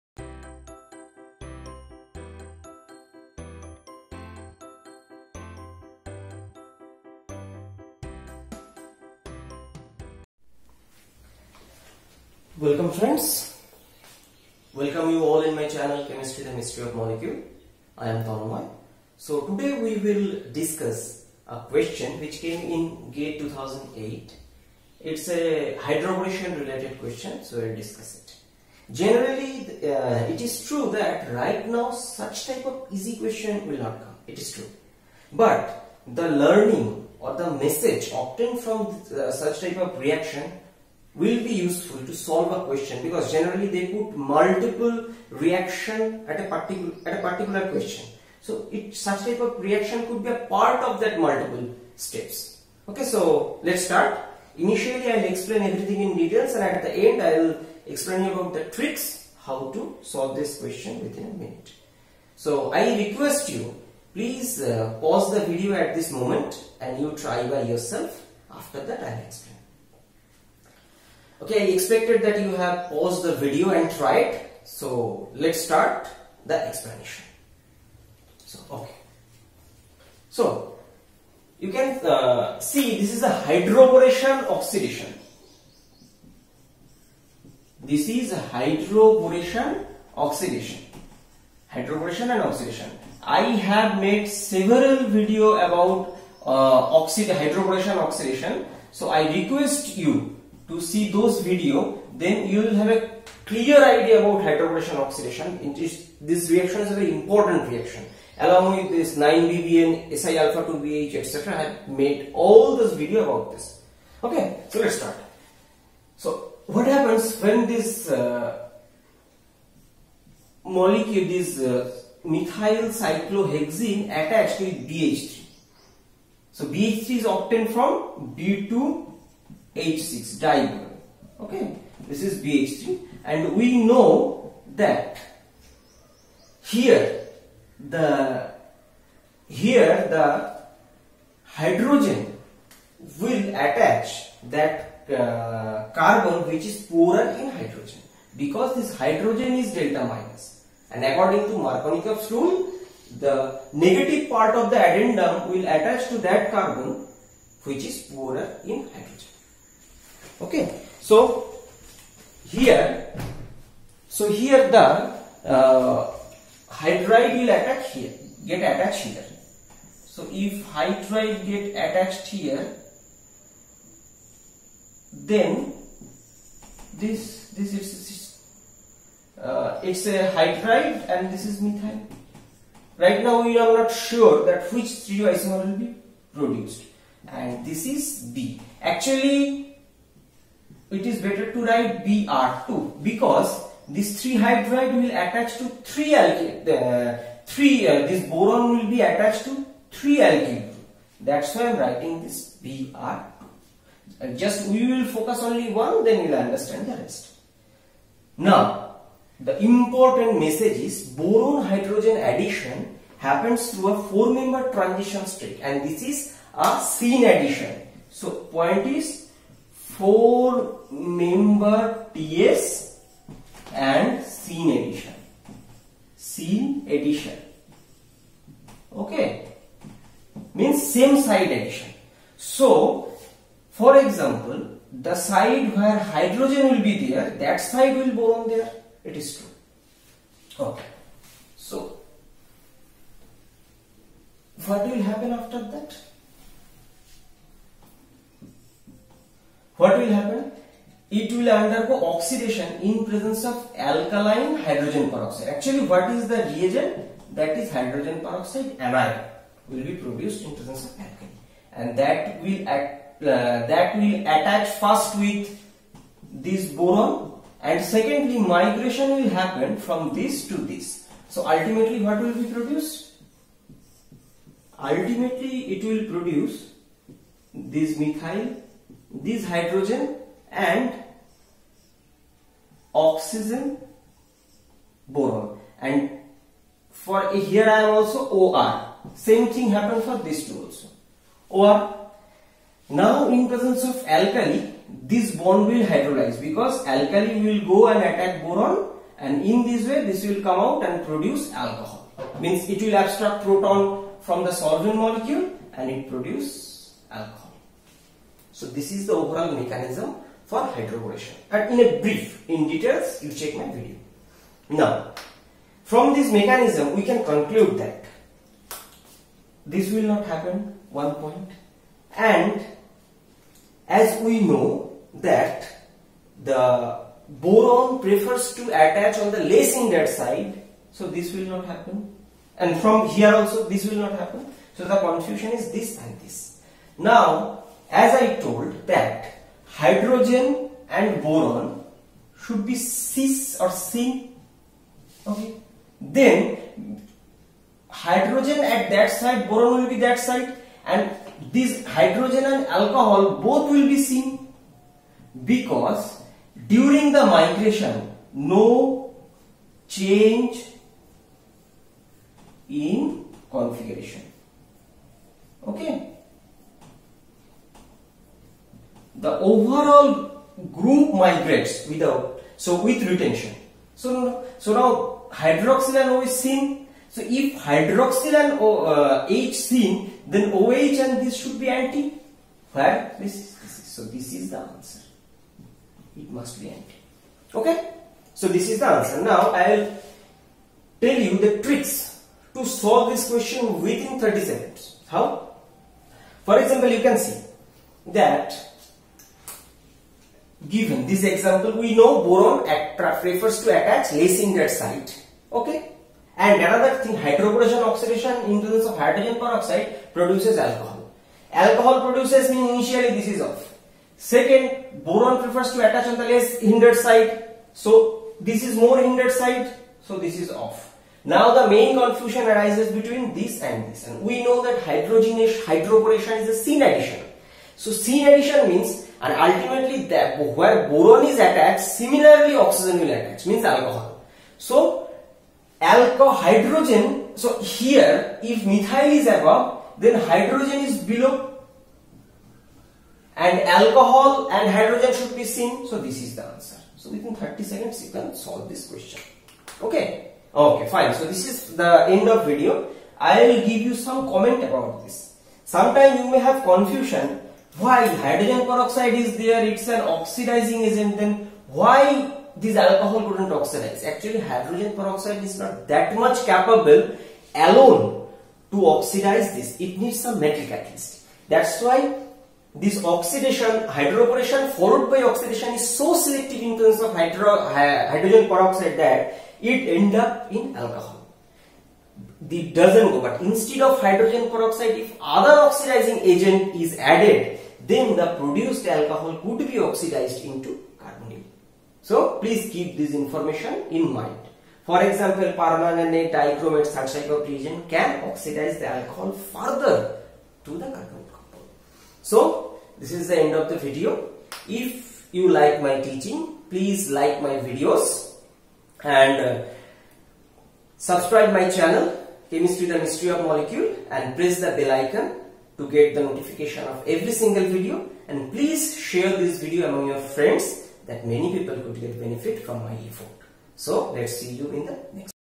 GATE-2008, Question Answer discussion about Hydroboration of methylcyclohexene,
In this video, I have discussed about Hydroboration of methylcyclohexene its selectivity (both regioselectivity and stereoselectivity). Such concept may be important for the exams like CSIR-NET, GATE, JAM, IIT-JEE, NEET, etc. exams.